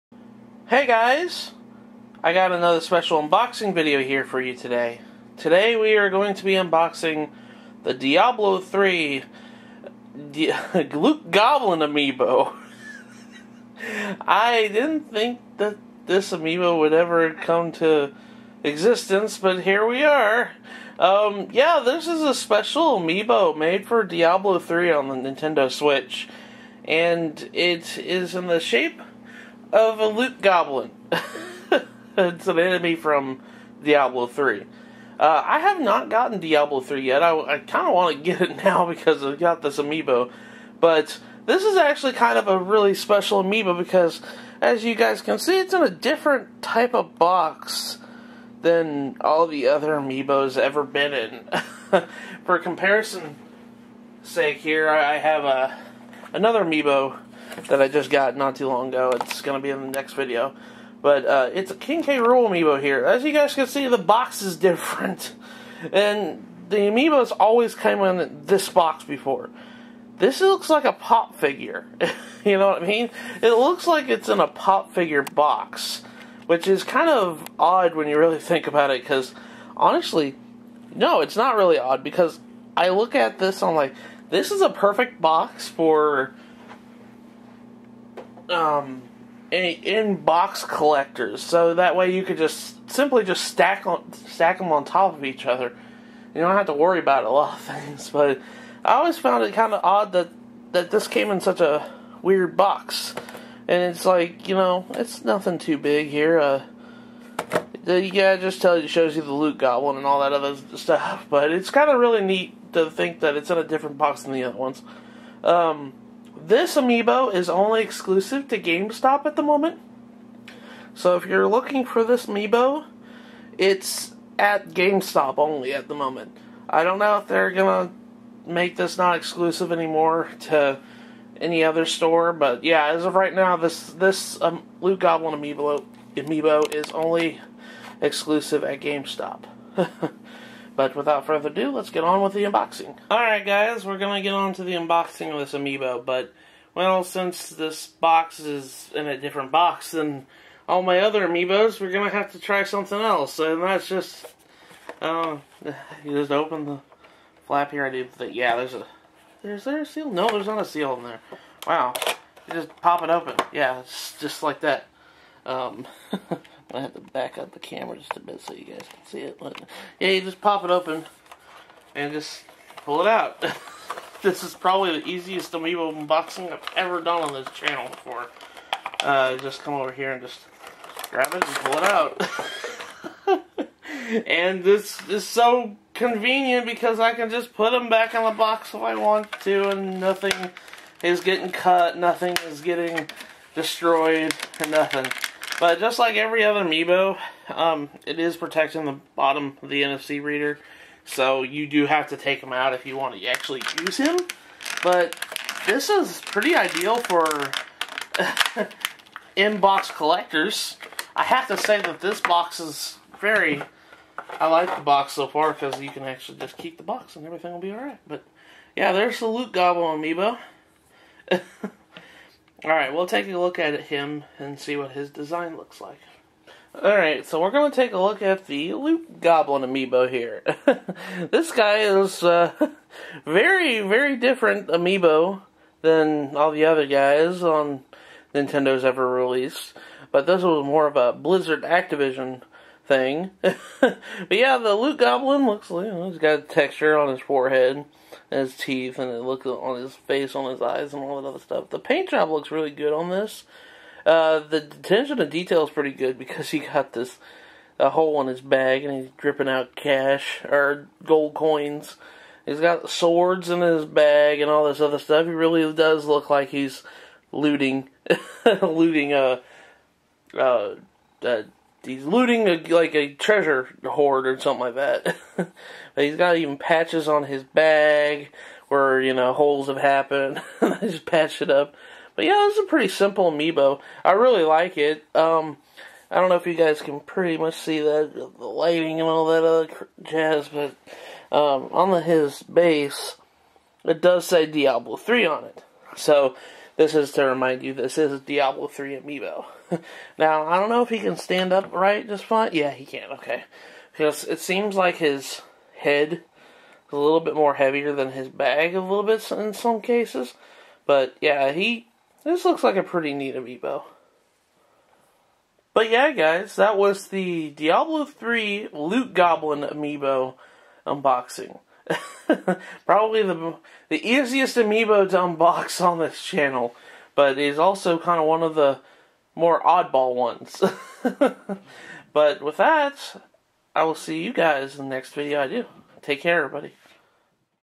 Diablo 3: Eternal Collection Loot Goblin Amiibo Unboxing
Today I unbox another 3rd Party Amiibo this time from the Diablo Series! Its a Loot Goblin!